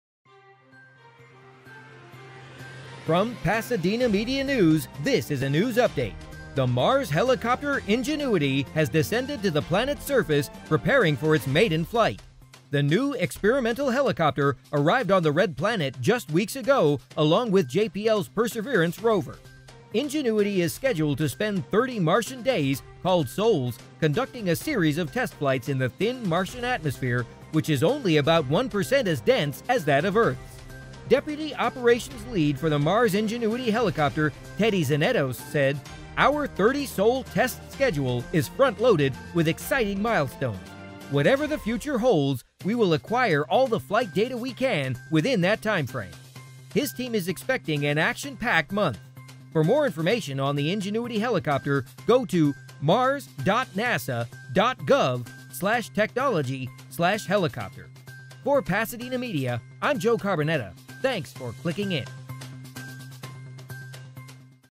PMN NASA JPL Mars Ingenuity Helicopter
The Ingenuity Mars Helicopter has descended on to the planet’s surface ahead of its anticipated maiden flight.